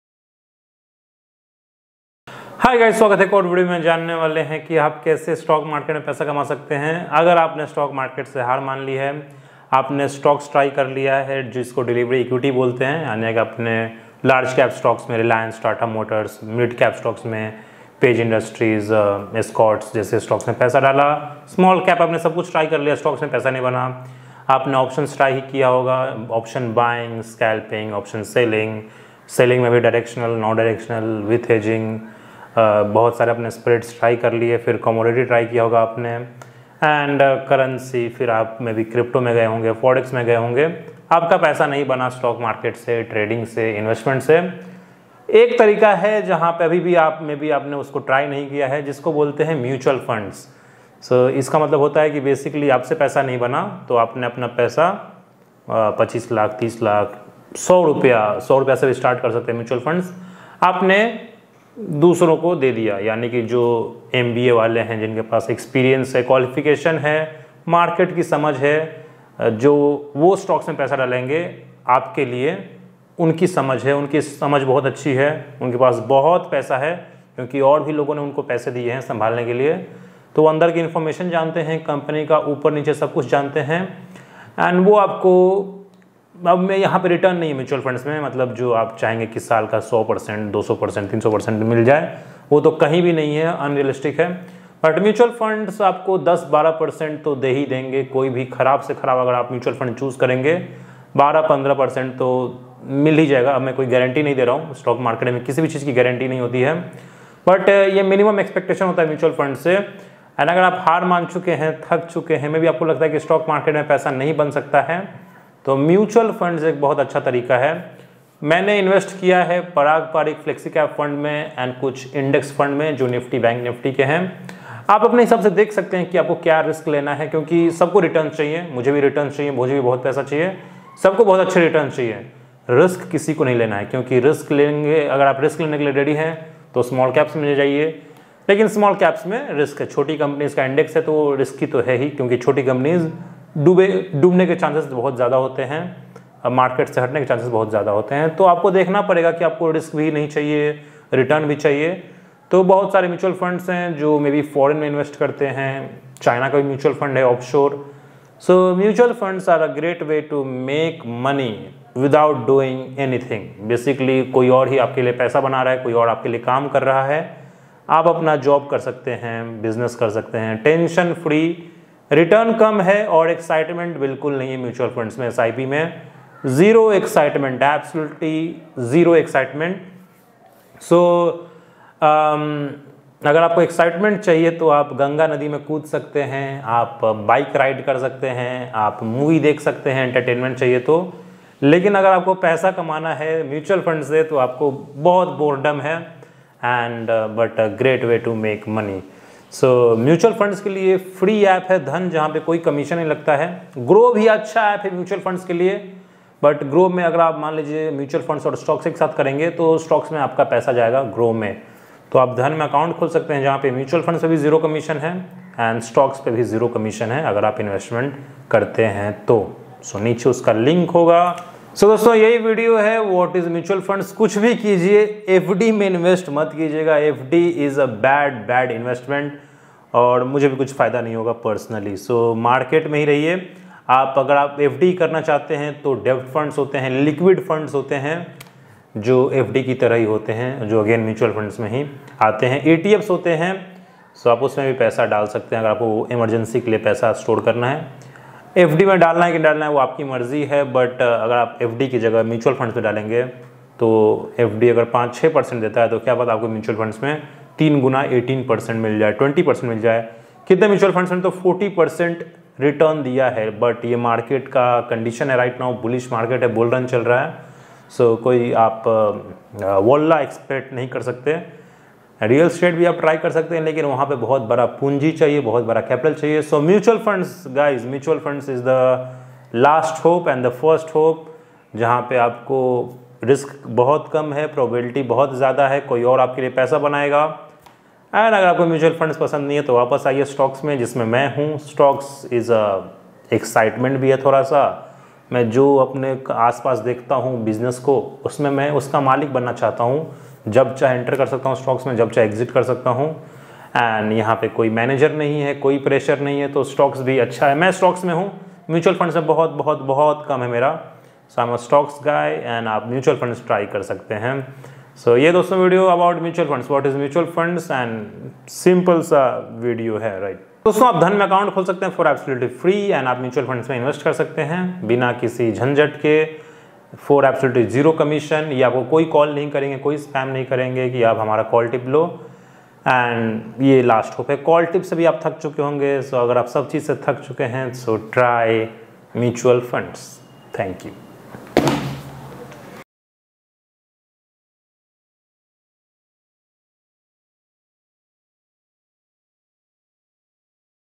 Quitting Stock Market ? Last and Best Way to Make Money in Stocks Market 🥇
🎁Start Investing in Mutual Funds for FREE, Open FREE Account in DHAN TODAY

✅ AFTER FIRST TRADE SEND SCREENSHOT ON

Explore the world of mutual funds with our latest video designed specifically for beginners!

In this comprehensive guide, we break down the essentials of mutual funds, highlighting why they are often a better investment choice compared to futures options and direct equity investments.

### Why Choose Mutual Funds?

- **Professional Management**: Mutual funds are managed by experienced professionals who make informed investment decisions on your behalf, reducing the burden of managing your portfolio.

- **Diversification**: By pooling money from many investors, mutual funds invest in a diverse range of assets, which helps mitigate risk. This is particularly advantageous for beginners who may not have the expertise to diversify their investments effectively.

- **Lower Risk**: Unlike trading in futures or individual stocks, mutual funds typically have a lower risk profile. They provide a more stable investment option, especially for those new to investing.

- **Accessibility**: With a variety of mutual fund types available, from equity to debt and hybrid funds, beginners can easily find options that align with their financial goals and risk tolerance.

### What You Will Learn:

- The best mutual fund categories for beginners to kickstart their investment journey.

- Key factors to consider when selecting mutual funds, including your investment goals and risk appetite.

- Insights into the performance of top-performing mutual funds and how they can help you build wealth over the long term.

Join us as we demystify mutual funds and empower you to make informed investment decisions.